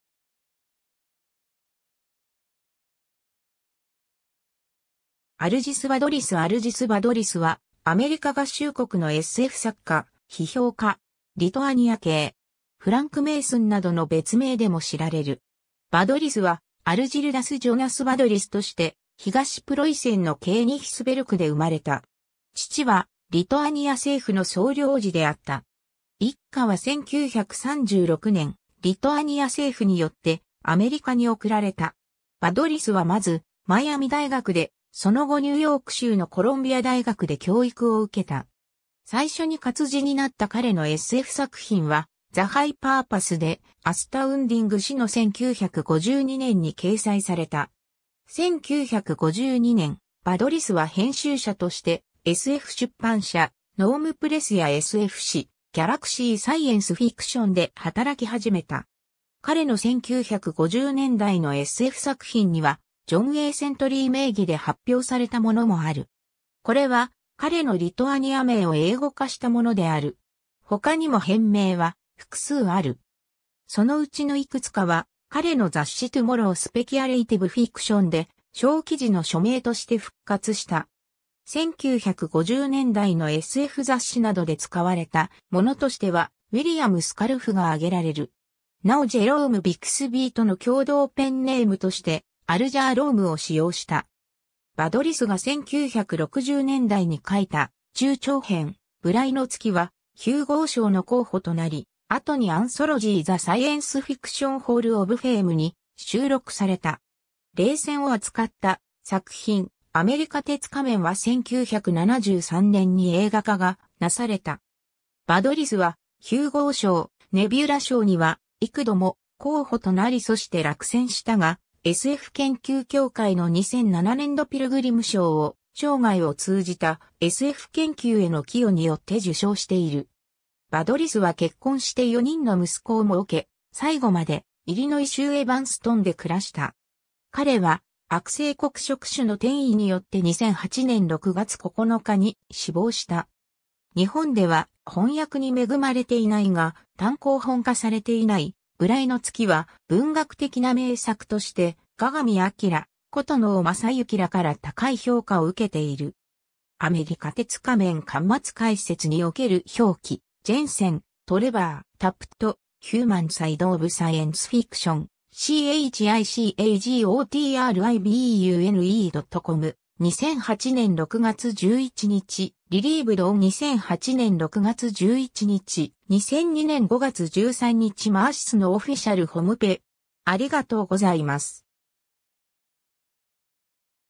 アルジス・バドリス
アルジス・バドリス（1985年の写真） アルジス・バドリス（Algis Budrys、1931年1月9日 - 2008年6月9日）はアメリカ合衆国のSF作家、批評家。
リトアニア系。
フランク・メイスン（Frank Mason）などの別名でも知られる。
 バドリスはアルジルダス・ジョナス・バドリス（Algirdas Jonas Budrys）として東プロイセンのケーニヒスベルクで生まれた。
父はリトアニア政府（当時の政府は戦後までアメリカに承認されていたとはいえ、バドリスの生涯を通じてソ連の傀儡政府が力を持ち続けた）の総領事であった。
一家は1936年（バドリスが5歳の時）、リトアニア政府によってアメリカに送られた。
 バドリスはまずマイアミ大学で、その後ニューヨーク州のコロンビア大学で教育を受けた。
最初に活字になった彼のSF作品は"The High Purpose"で、「アスタウンディング」誌の1952年に掲載された。
1952年、バドリスは編集者としてSF出版社ノーム・プレスやSF誌「ギャラクシー・サイエンス・フィクション」で働き始めた。
彼の1950年代のSF作品には、ジョン・A・セントリー（John A. Sentry）名義で発表されたものもある。
これは彼のリトアニア名を英語化したものである。
他にも変名は複数ある。
そのうちの幾つかは彼の雑誌"Tomorrow Speculative Fiction"で、小記事の署名として復活した。
1950年代のSF雑誌などで使われたものとしては、ウィリアム・スカルフ（William Scarff）が挙げられる。
なおジェローム・ビクスビイ（Jerome Bixby）との共同ペンネームとしてアルジャー・ローム（Alger Rome）を使用した。
 バドリスが1960年代に書いた中長編『無頼の月』（Rogue Moon）はヒューゴー賞の候補となり（受賞はせず）、後にアンソロジー"The Science Fiction Hall of Fame"（1973年）に収録された。
冷戦を扱った作品『アメリカ鉄仮面』（Who?）は1973年に映画化がなされた。
バドリスはヒューゴー賞、ネビュラ賞には幾度も候補となりそして落選したが、SF研究協会（Science Fiction Research Association）の2007年度ピルグリム賞（Pilgrim Award）を「生涯を通じたSF研究への寄与」によって受賞している。
 バドリスは結婚して4人の息子をもうけ、最期までイリノイ州エヴァンストン（Evanston）で暮らした。
彼は悪性黒色腫の転移によって2008年6月9日に死亡した。
 日本では翻訳に恵まれていないが、単行本化されていない『無頼の月』は文学的な名作として、鏡明、殊能将之らから高い評価を受けている。
 ^ 『アメリカ鉄仮面』巻末解説（仁賀克雄）における表記 ^ ^ Jensen, Trevor. "Tapped human side of science fiction", chicagotribune.com, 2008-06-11. Retrieved on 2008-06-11. ^ 2002年5月13日 - mercysnow official homepage (Internet Archive)